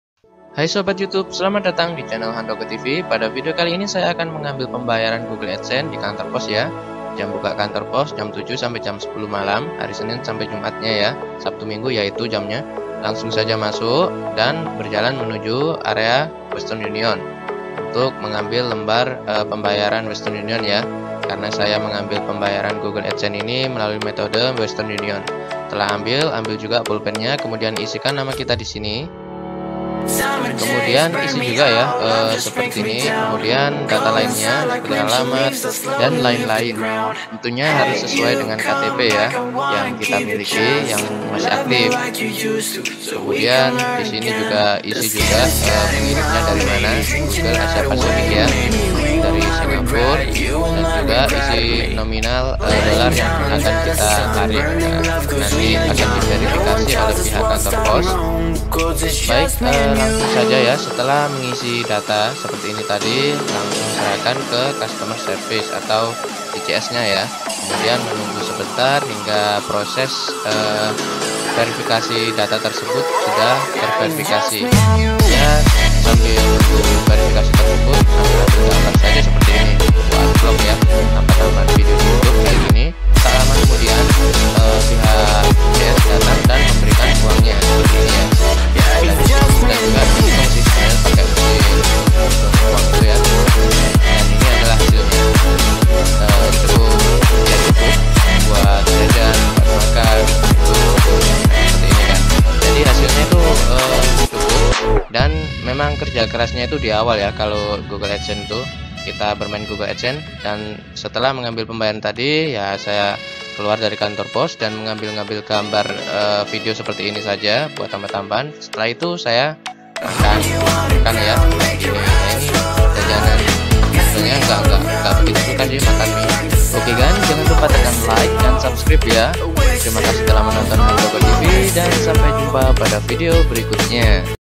Cara mudah raih uang dari Adsense
Copyright Yaser Khamil

Kali ini saya akan menampilkan video baru dengan judul Cara mudah raih uang dari Adsense .
Video ini sangat baik anda lihat kika anda mempunyai atau yang rencana mau buat akun adsense, karena membuat kita tahu lebih banyak tentang adsense. Selamat menonton guys...